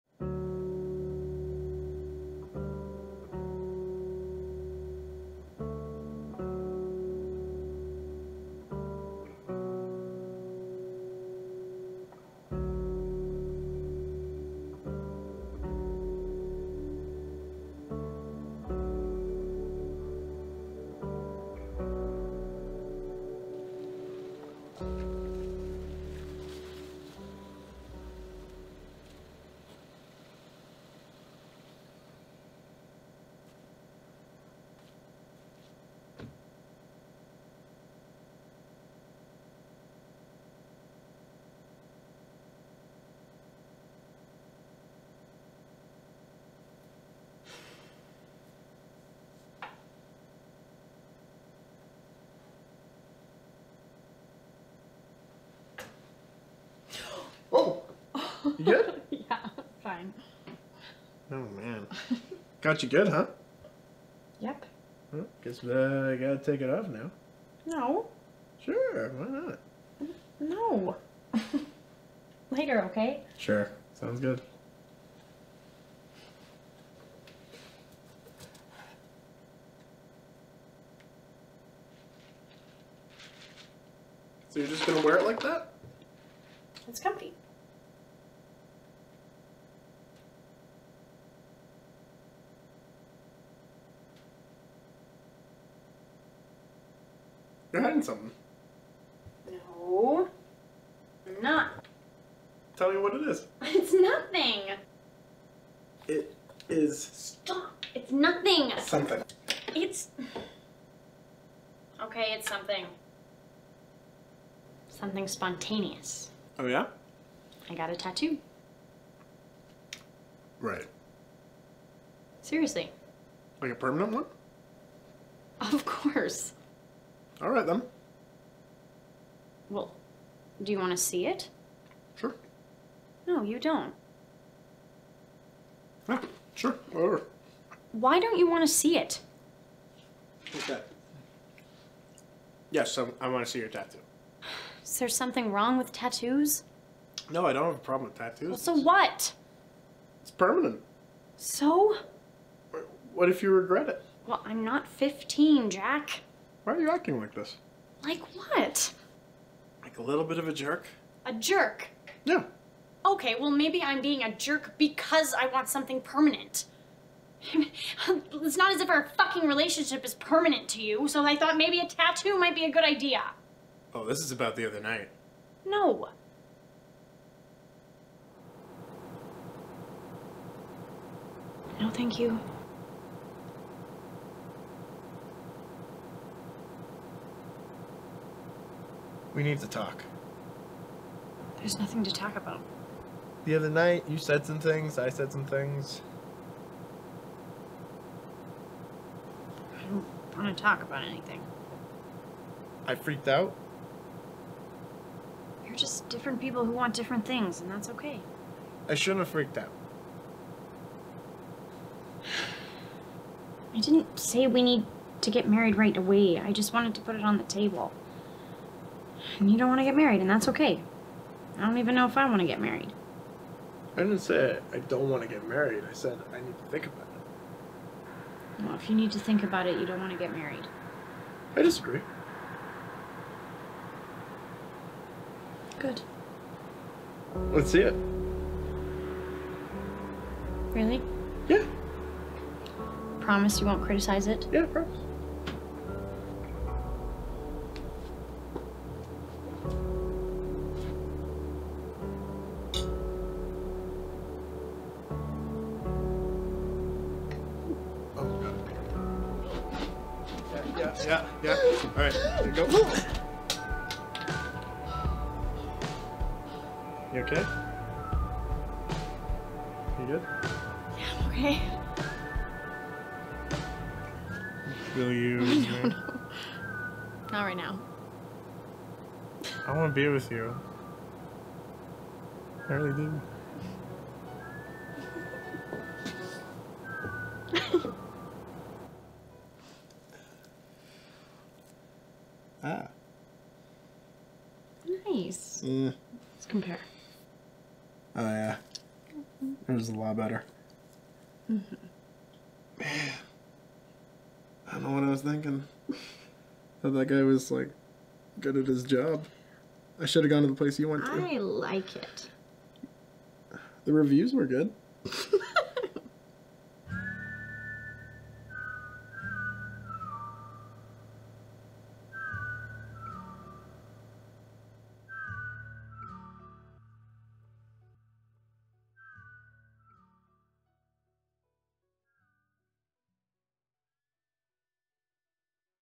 Hiding a new tattoo from your partner | Short Film
A spontaneous woman reveals her first tattoo to her by-the-books boyfriend, which ends up exposing a bigger problem in their relationship.

BACKSTORY:
I shot this short film in February 2019 and sat on it for almost a year. I had pretty much no experience making short films. I had no idea how to rotoscope this window that's in it for 7 seconds to make it look like daylight and I used that as an excuse. But with it being almost a year later, I realized I am not perfect, I'm still learning, and ultimately it's not fair to the other people who worked on this for me to sit on it like a perfectionist. So I decided to release it to the world as-is and hold it up as a great learning experience for me.
EQUIPMENT USED:
Camera: Sony FS700 with Rokinon prime lenses
Microphone: Rode NTG 2